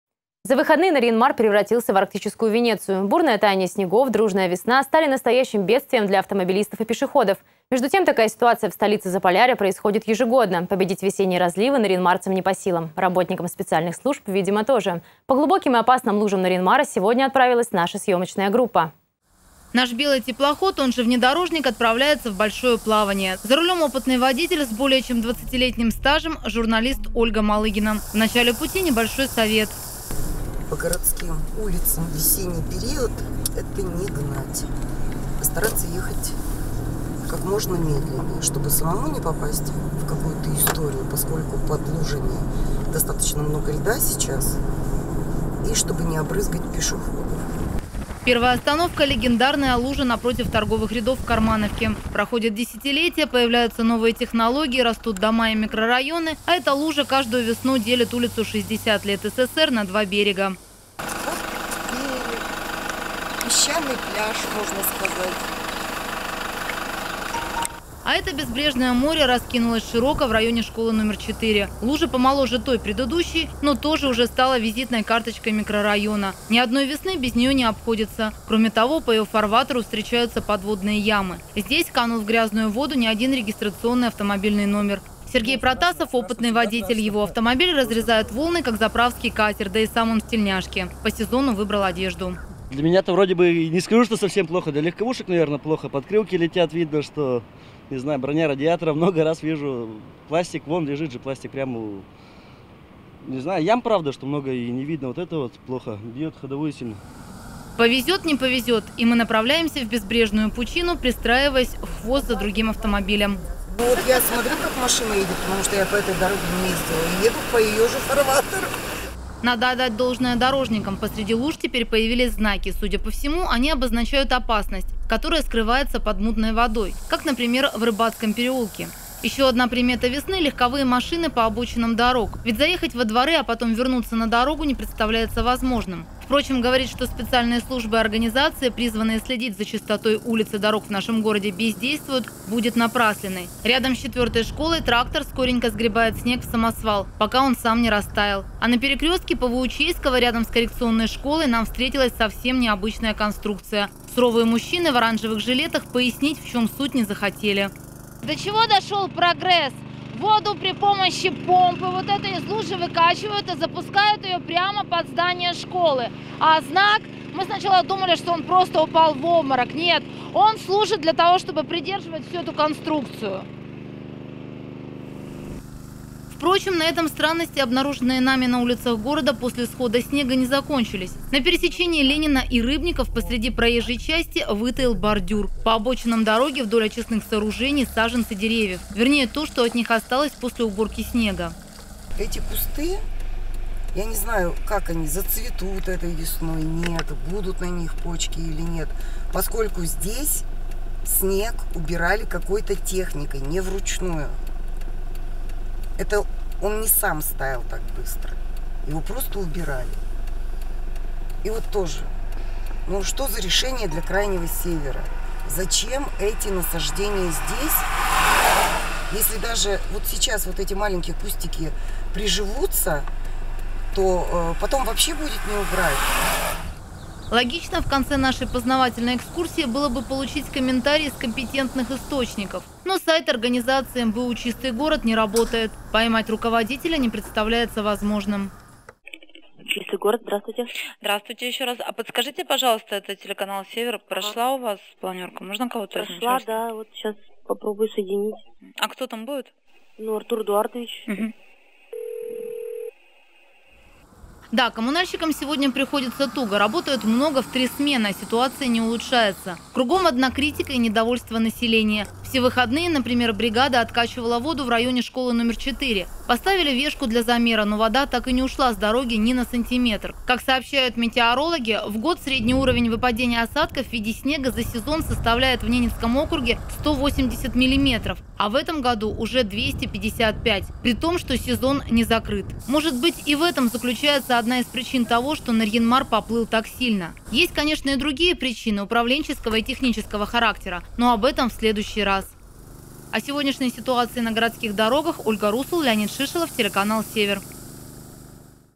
Непроходимые лужи образовались на городских дорогах в результате таяния снега
За выходные Нарьян-Мар превратился в Арктическую Венецию. Обильное таяние снегов и ранняя весна обернулись настоящим бедствием для автомобилистов и пешеходов. Между тем, такая ситуация в столице Заполярья происходит ежегодно.